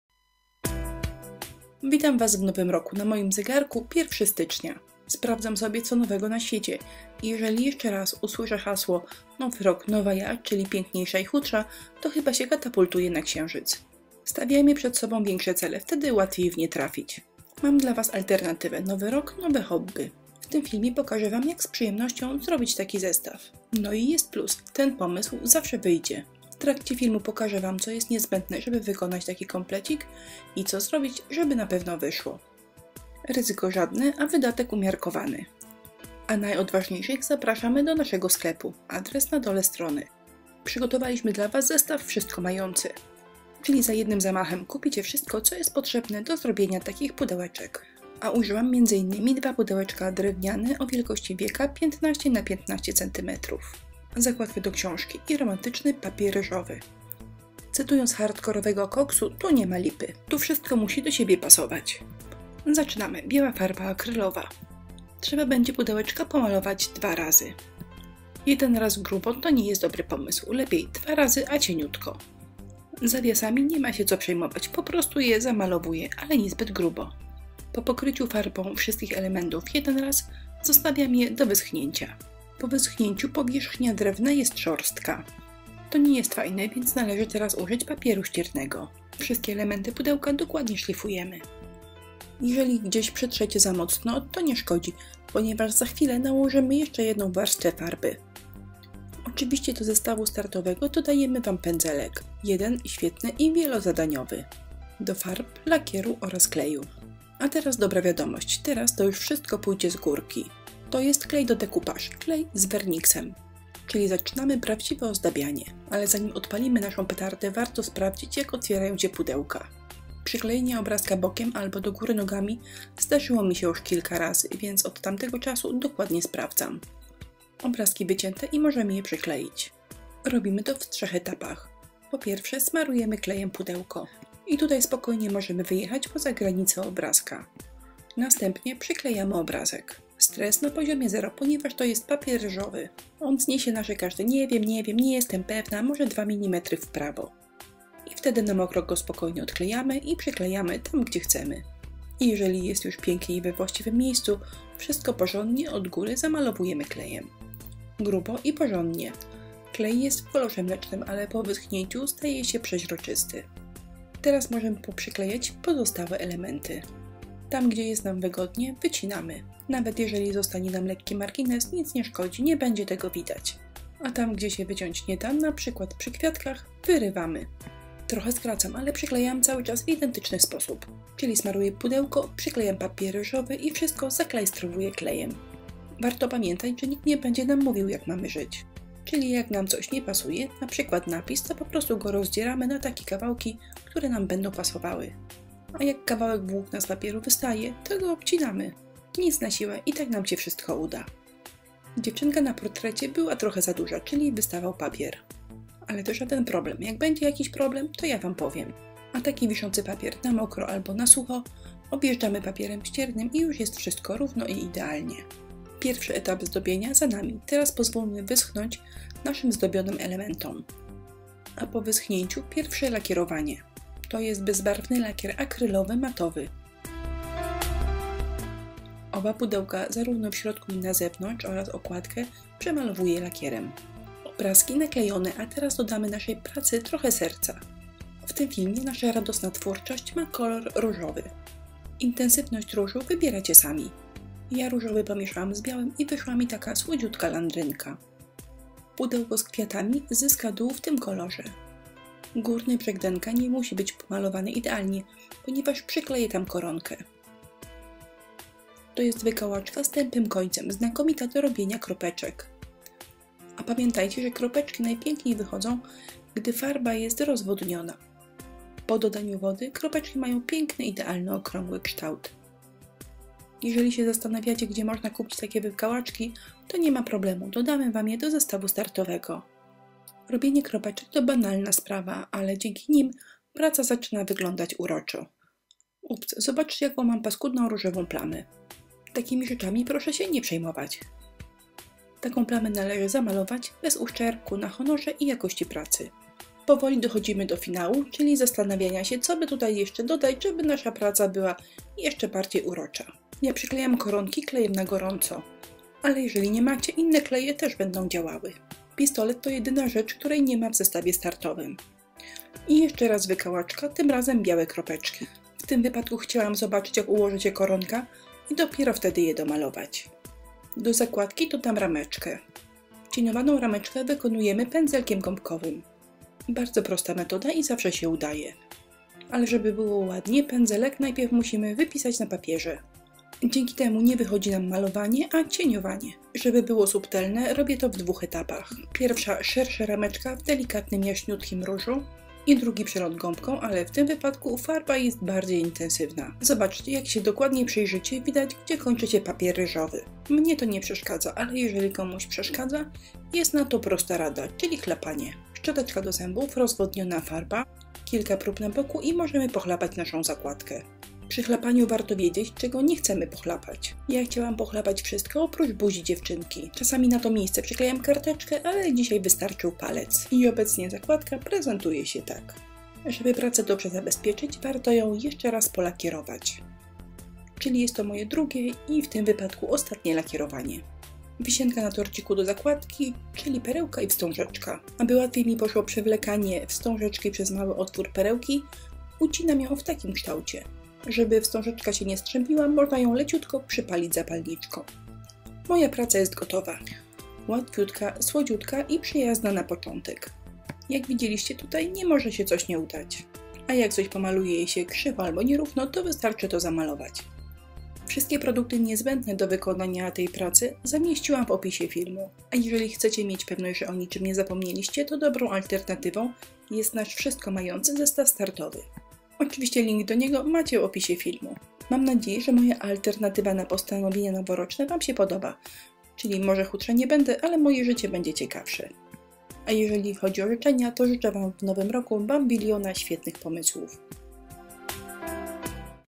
Zestaw startowy romantyczny - DIY tutorial
Film przedstawia pomysł na Twoją pierwszą pracę w technice decoupage.  Prosty pomył i łatwe wykonanie gwarantują ładny efekt końcowy. Chcesz więcej? Zapraszamy na

W filmie użyłam poniższe produkty:
2x pudełko  15x15 cm:

Wstążeczka 1m

Wykałaczki - jeżeli potrzebujesz, napisz w komentarzu do zamówienia, dodamy je do paczki.